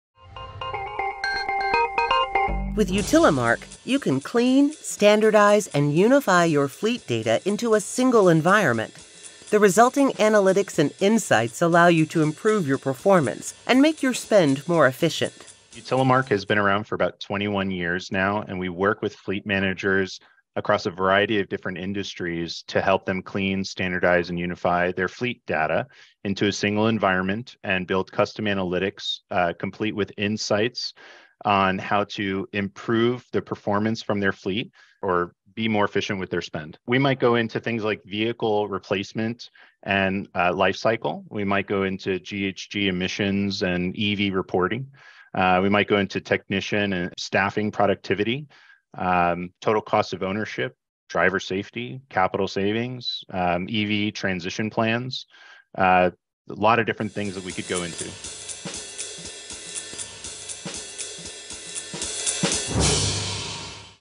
Fleet Data That Clearly Builds Efficiencies for Your Fleet | ENGIN AYAZ | Fleet Management Weekly
With Utilimarc you can clean, standardize and unify your fleet data into a single environment. The resulting analytics and insights allow you to improve your performance and make your spend more efficient.

ENGIN AYAZ
Sales Account Manager
Utilimarc